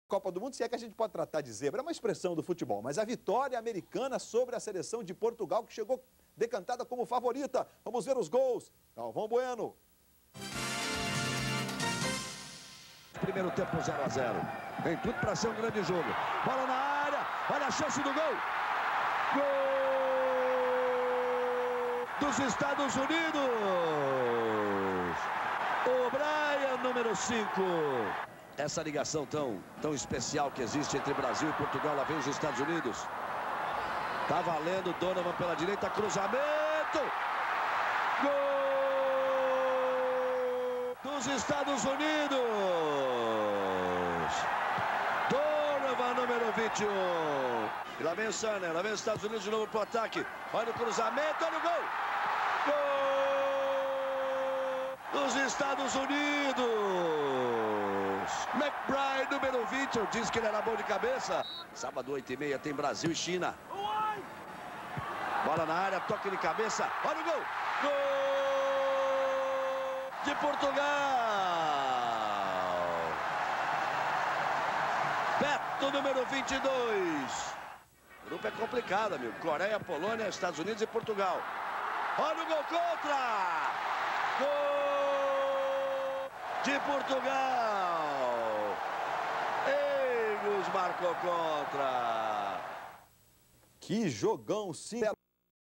GOLS EUA 3X2 PORTUGAL COPA 2002 GLOBO
Estréia das 2 seleções! Narração Galvão Bueno